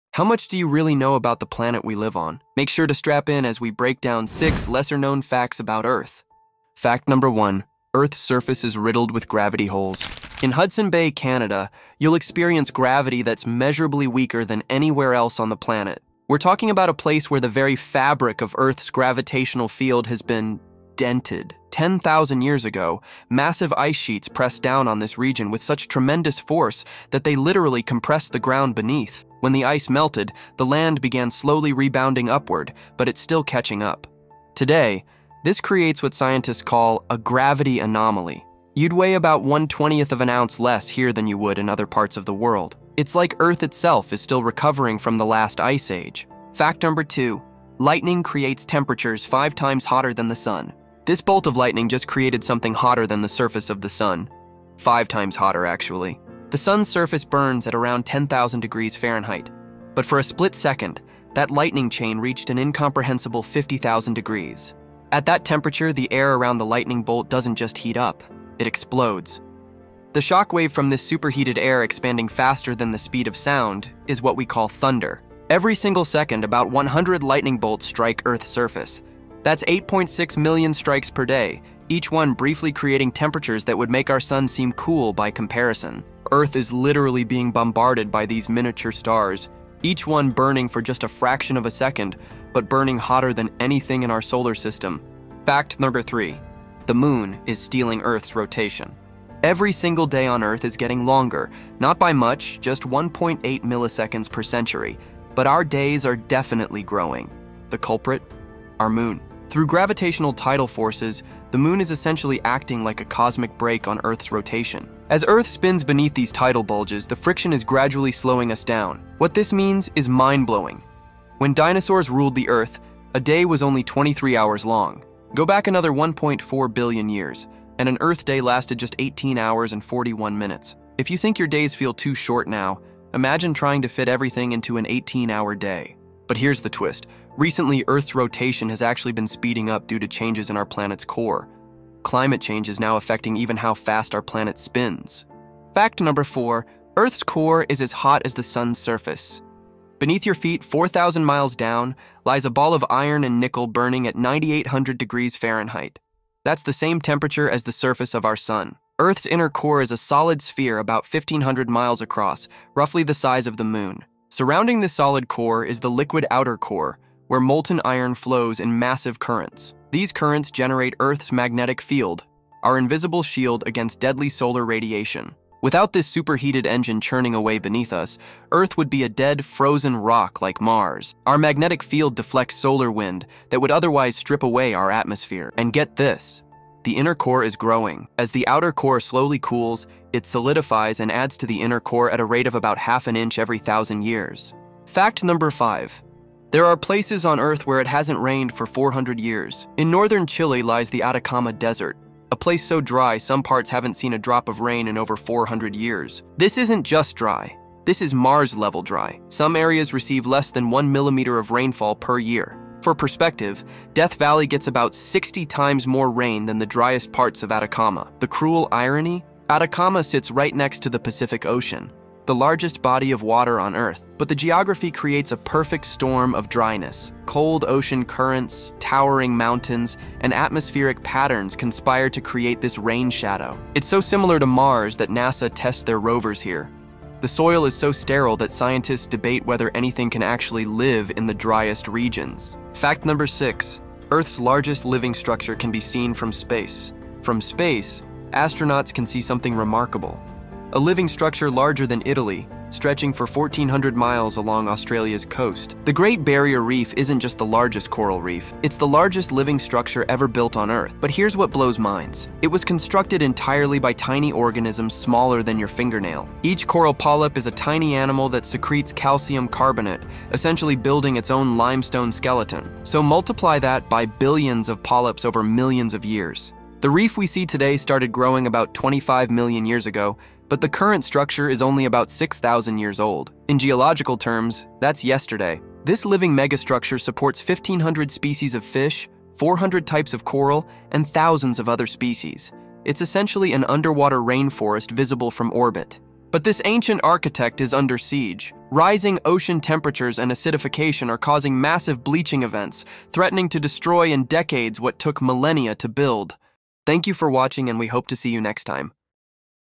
6 Facts You Didn’t Know About Earth
Discover the hidden secrets of Earth that most people never learn about! From gravity holes in Canada where you actually weigh less, to lightning that burns 5x hotter than the Sun’s surface. Did you know the Moon is literally stealing Earth’s rotation, making our days longer? Or that some places on Earth haven’t seen rain in 400 years?

Join us on an incredible journey through our planet’s most extraordinary mysteries - from the scorching core that matches the Sun’s temperature to ancient life that’s almost as old as Earth itself. You’ll never look at clouds, rocks, or even your compass the same way again.

Factoidbible.com

Sources: NASA, ESA, Livescience, MIT, Harvard.

Note: This video is for educational and informational purposes. We try our best to read every comment.